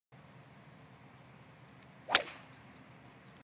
Tano Goya Driver front view
Tano Goya en la practica del Estoril Open de Portugal 2010